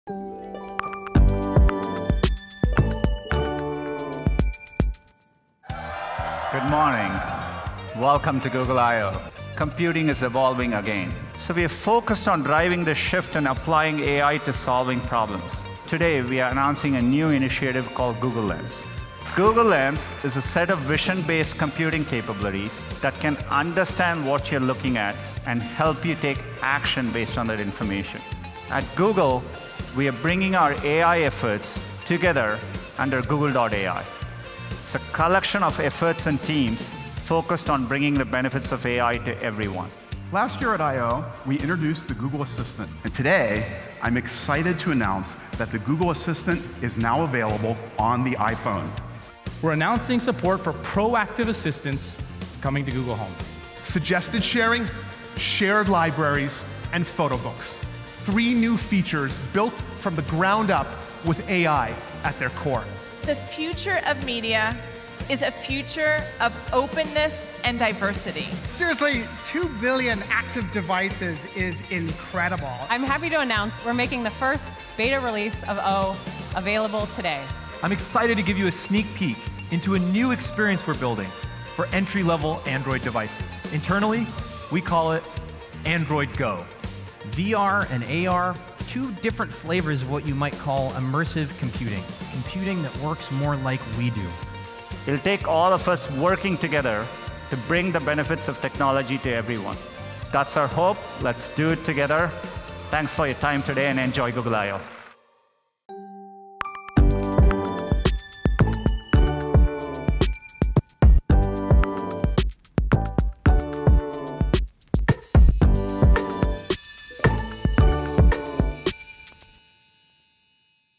Google Keynote Highlights (Google I/O '17)
Recap the highlights and top announcements of this year's Google Keynote led by Sundar Pichai.

Keynote and session recordings: g.co/io/videos
I/O Guide videos: g.co/io/guide
Event photos: g.co/io/photos
io17 event: Google I/O 2017; re_ty: Publish;